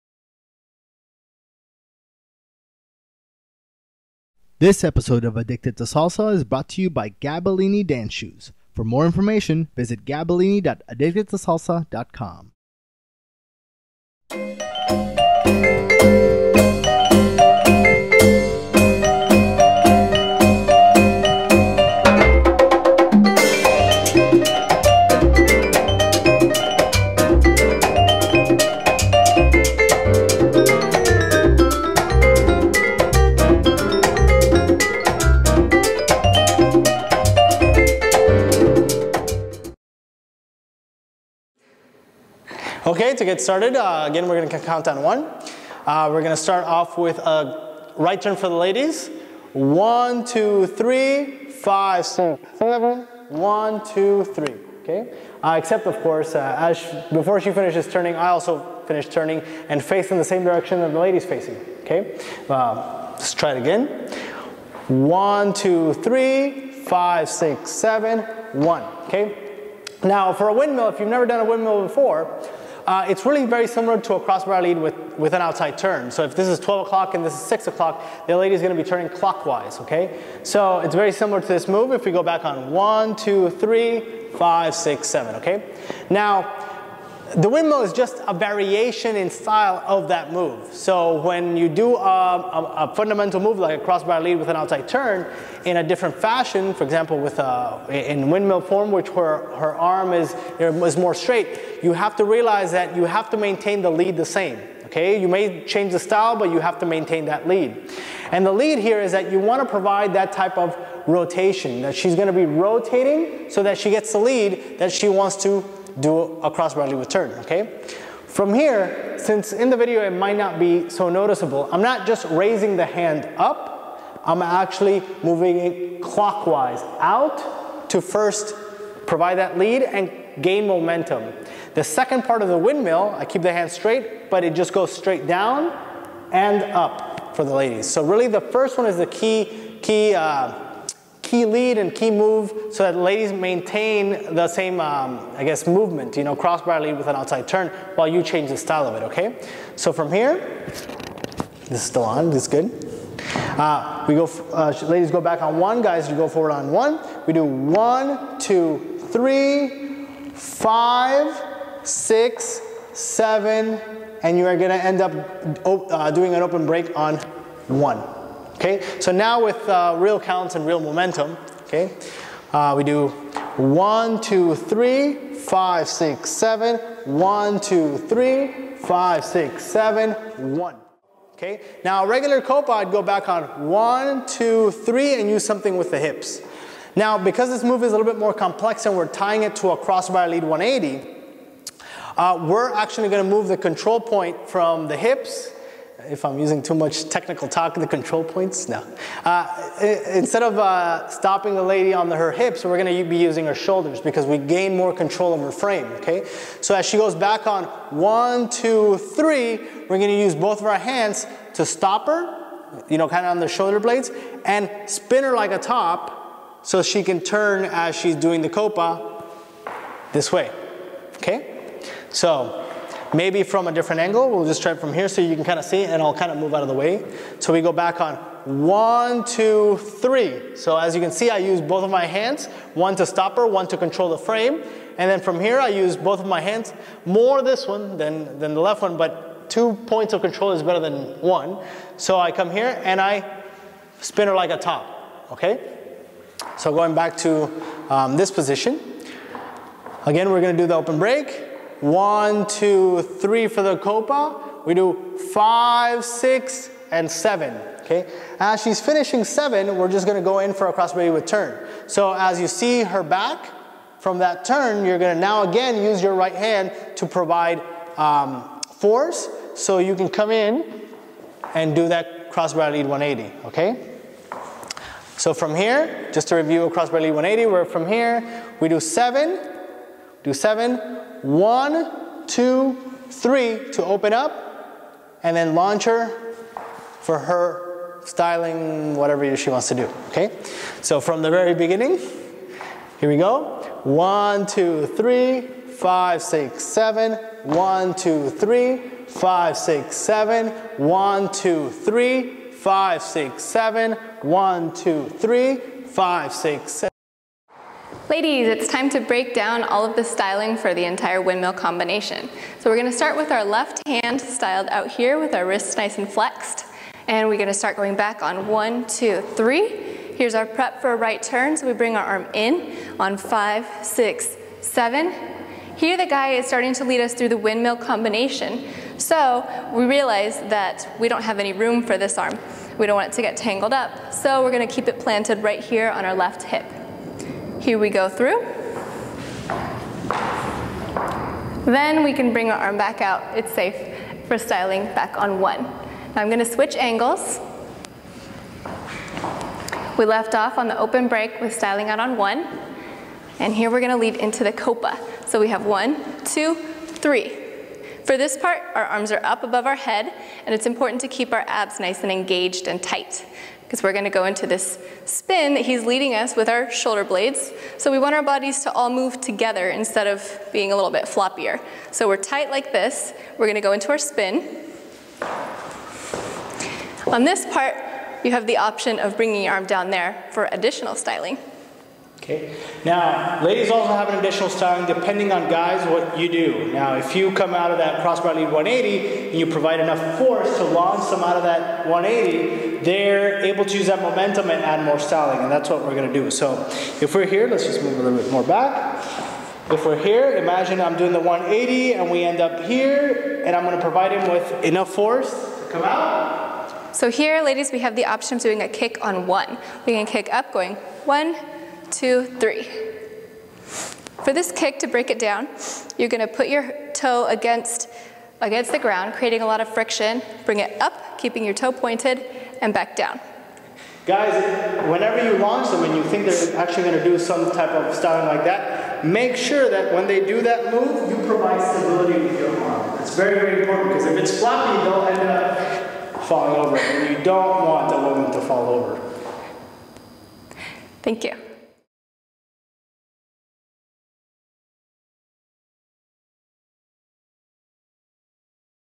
Salsa Dance : Windmill Pattern and Styling
This salsa dance class will show you how to dance the best of salsa dance windmill dancing moves - salsa windmill dance combination into an hourglass salsa dance copa. Julie will provide salsa dance style lessons for ladies. Hope you enjoy our salsa dance class series.